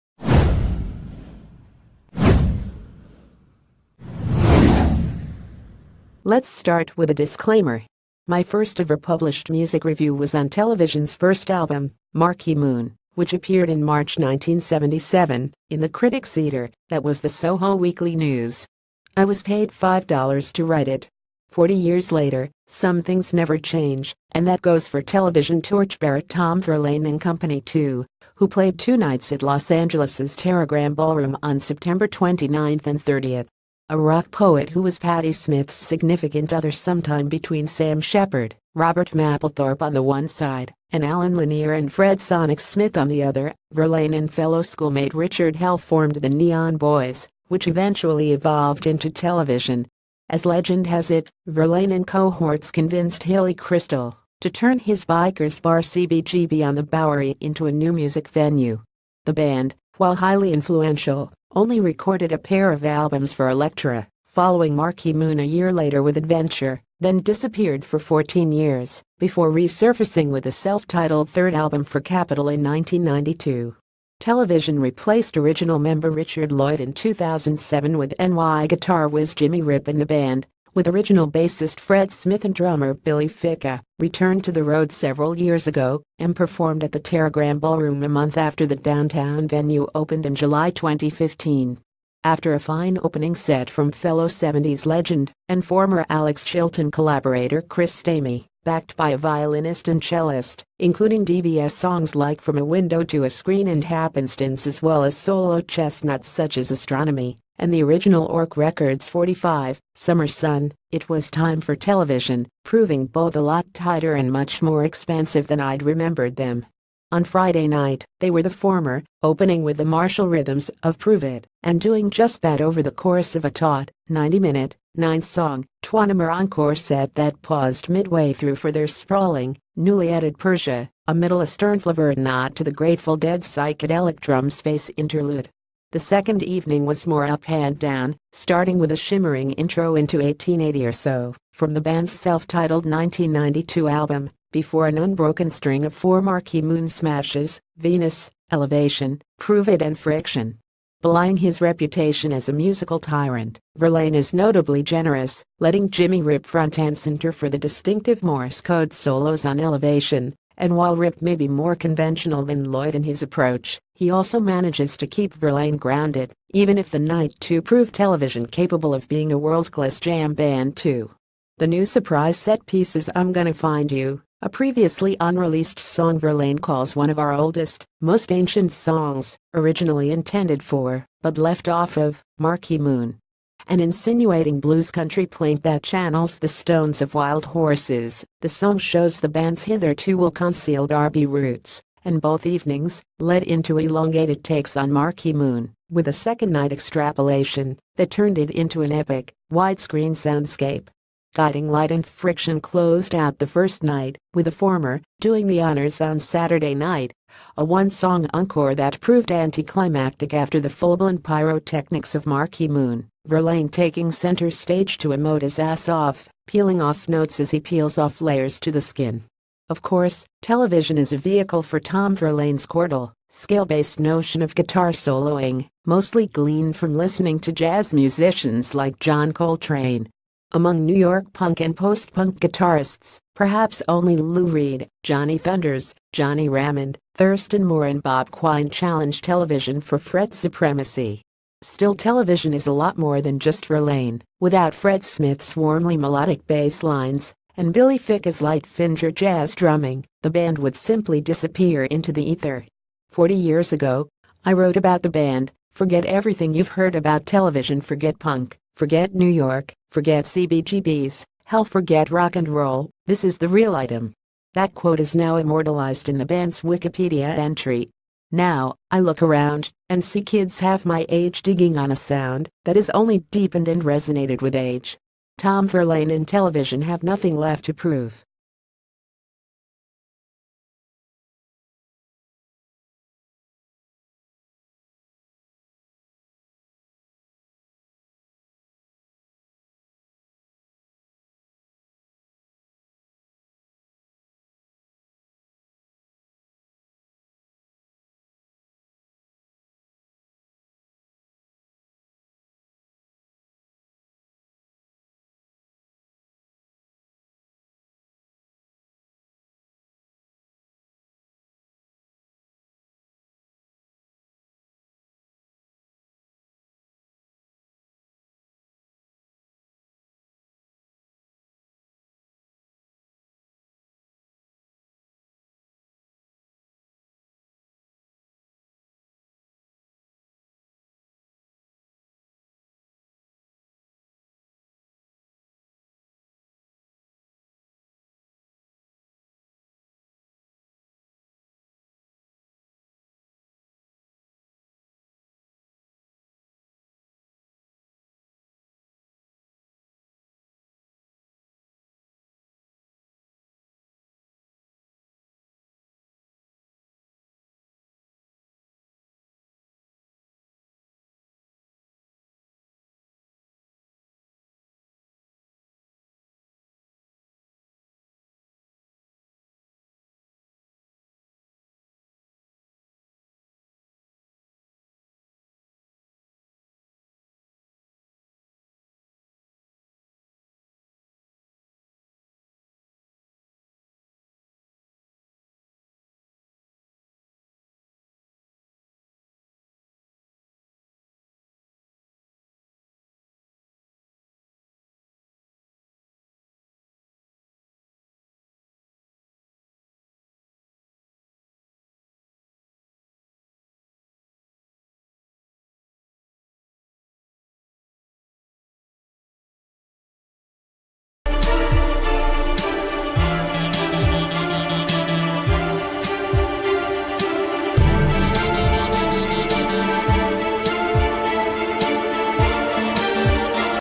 Breaking News | Concert review: revisiting the golden age of television from the left coast
Concert review: revisiting the golden age of television from the left coast